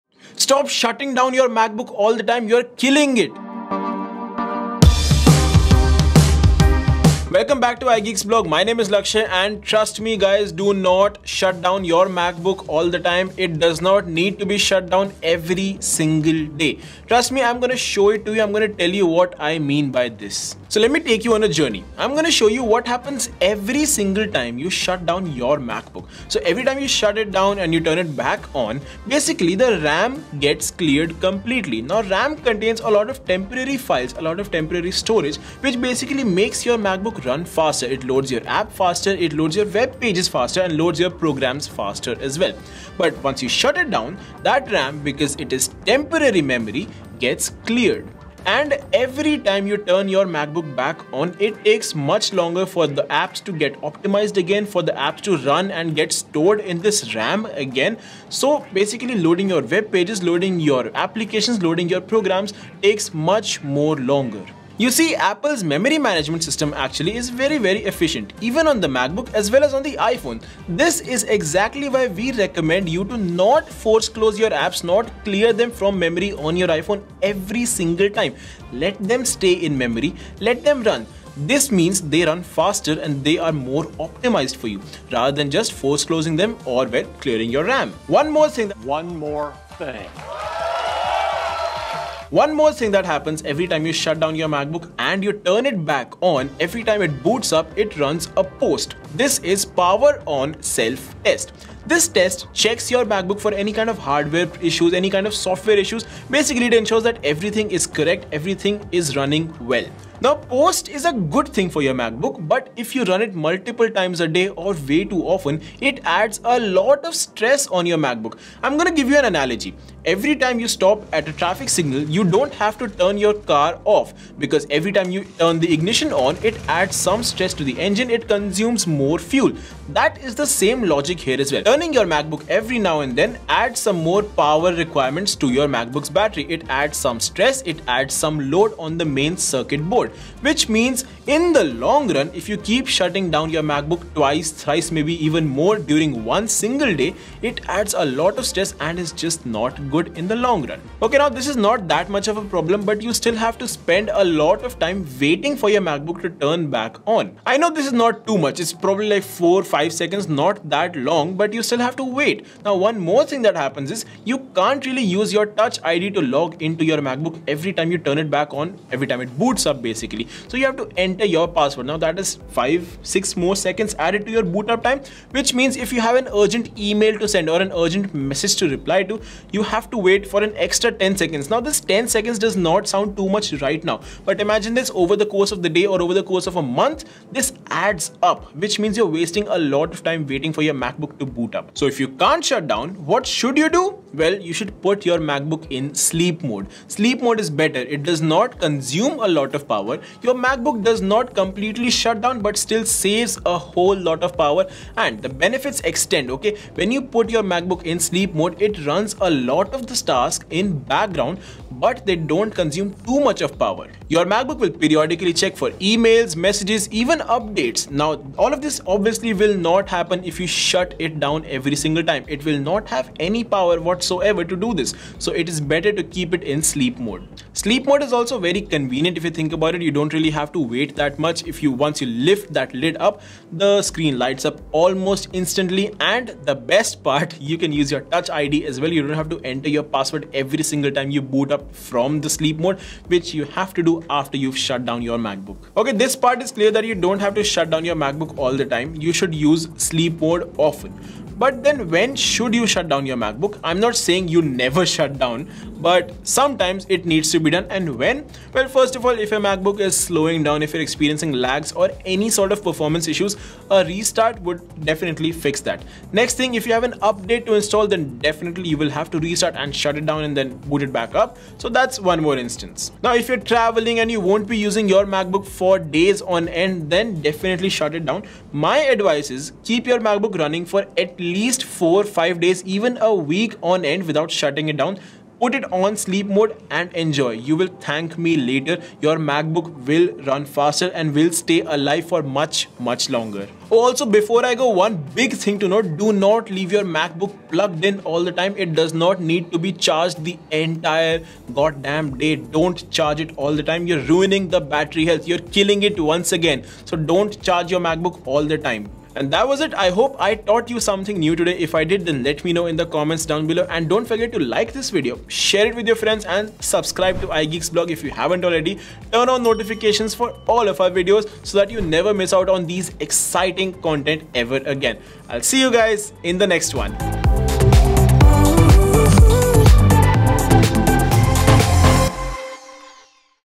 DON'T SHUT DOWN YOUR MACBOOK! 🚫🛑
Do you always shut your MacBook down after using it? In this video, I will tell you why doing that is not good for your MacBooks and why you should stop doing that right now!